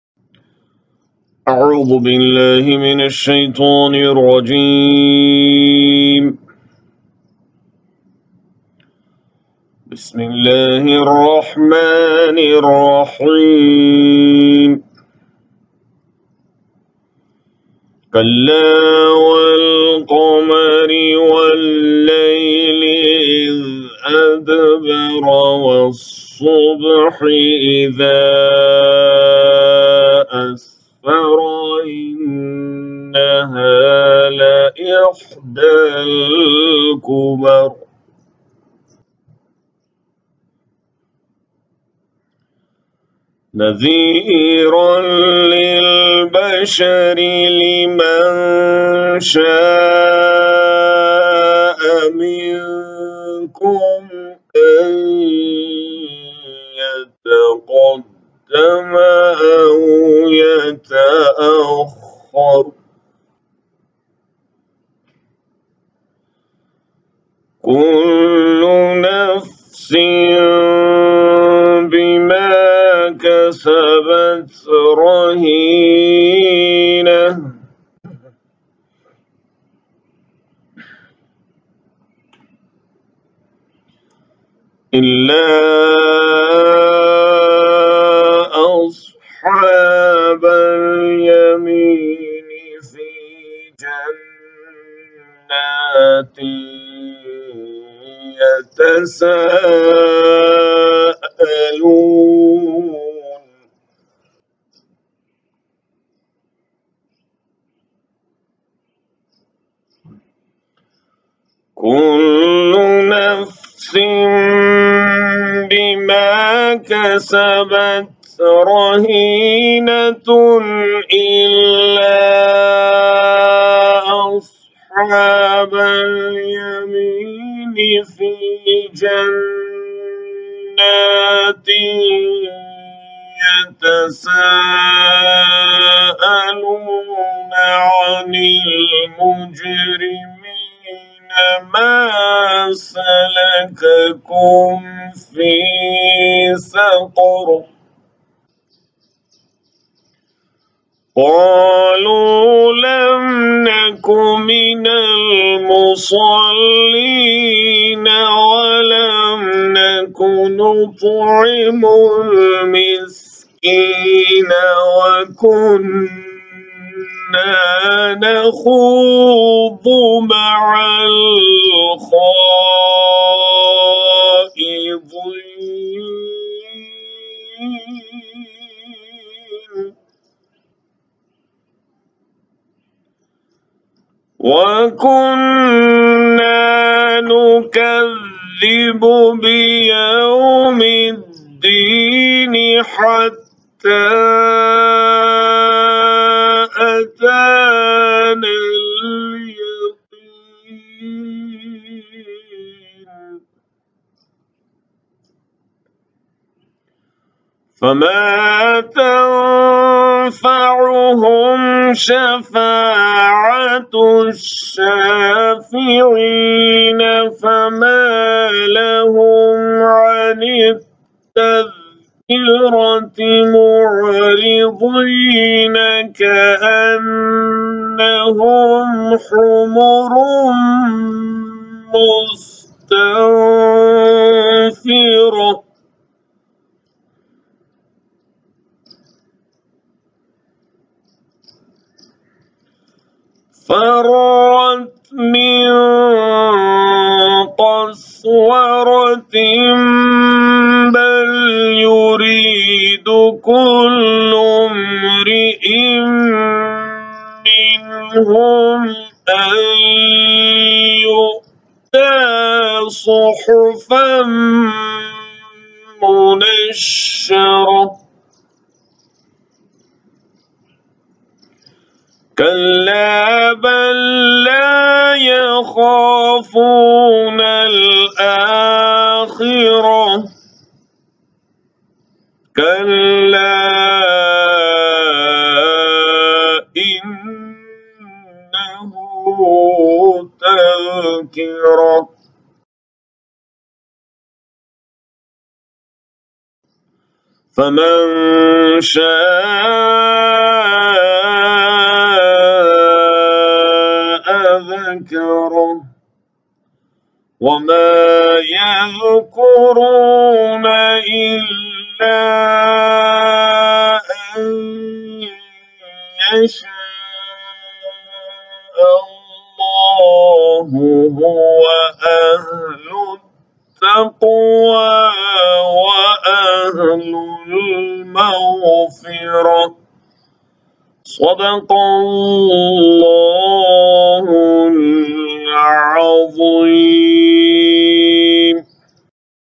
Suratul Muddassir Ayat 32-56
Suratul Muddassir Ayat 32-56  Tilawat by Qari Muhammed Abdul Mannan, Khateeb Masjid Hibatul Jabbar, Hyderabad, India